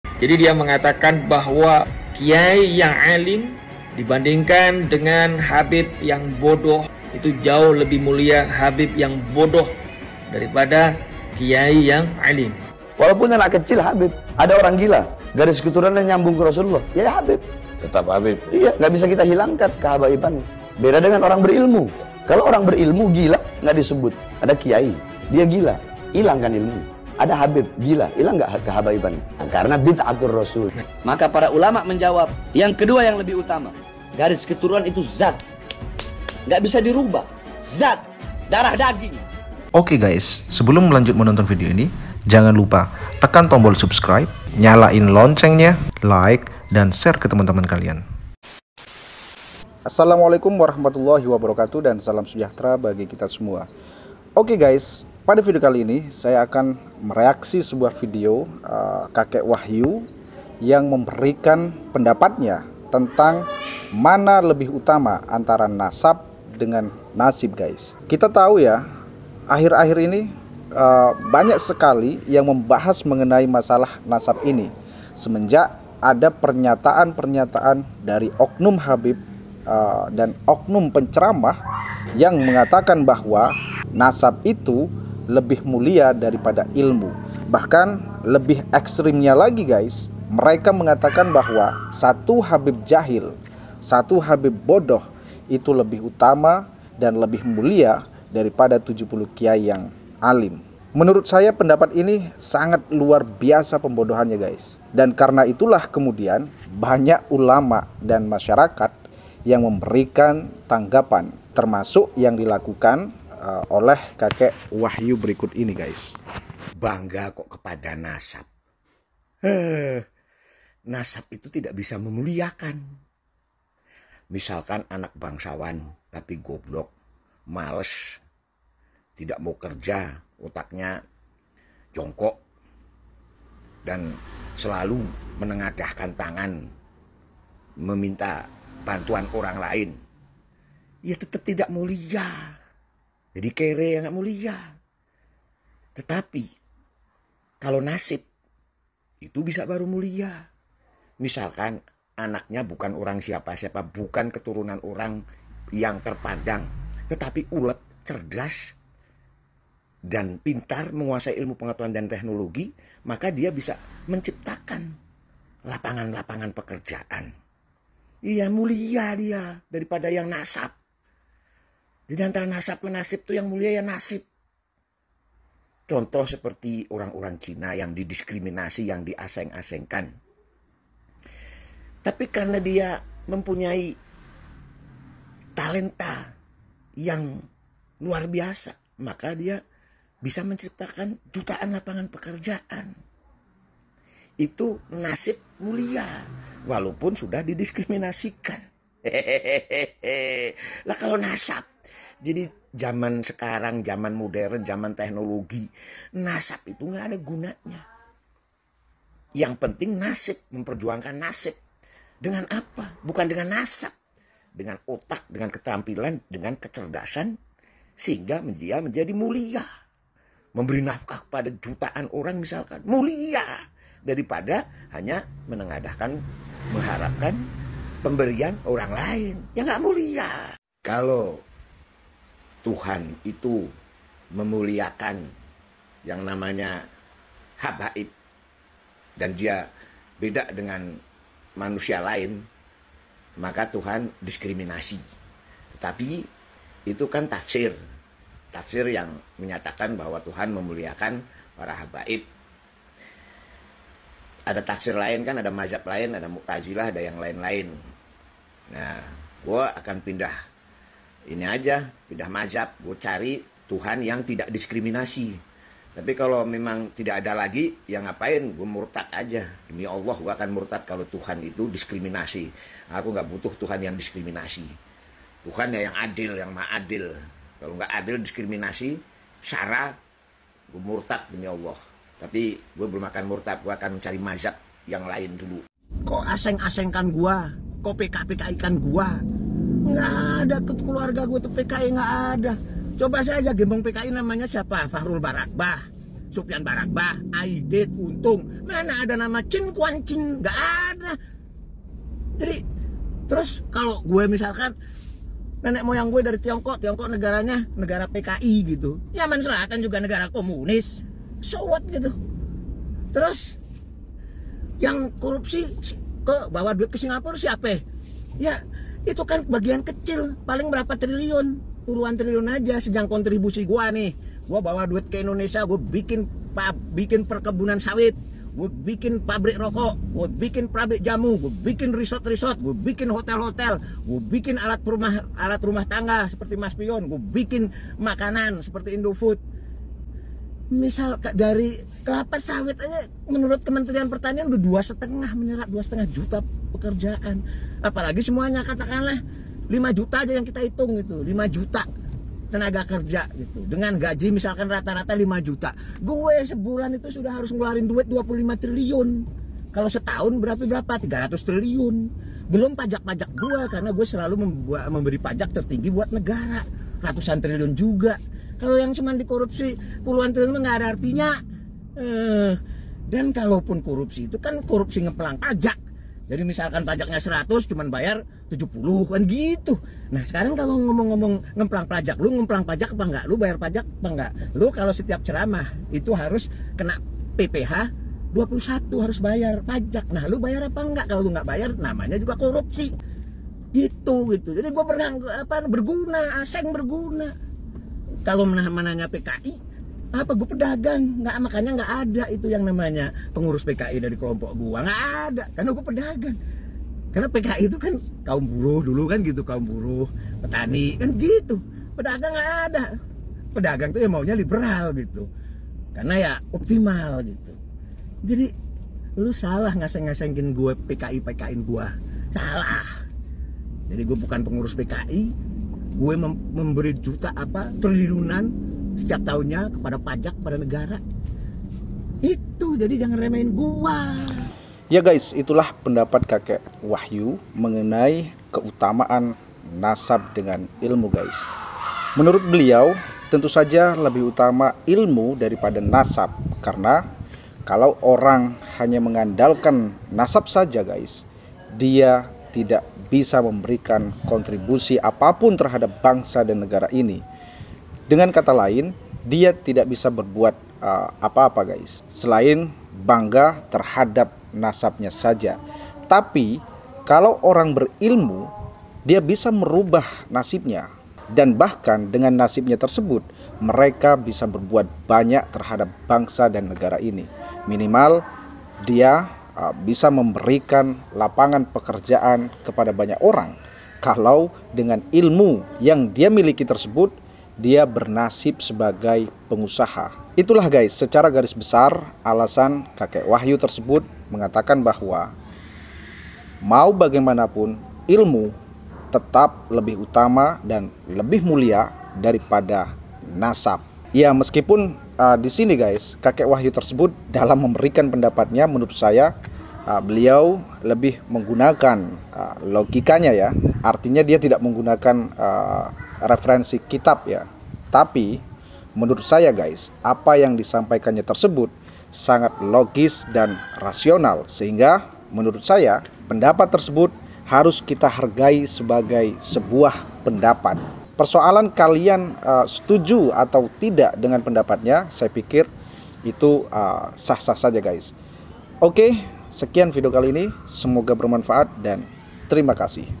JLEB‼️ NASEHAT KAKEK WAHYU KEPADA PARA KAUM SUDRUN
Nasehat kakek wahyu kepada kaum sudrun jangan sampai dilewatkan guys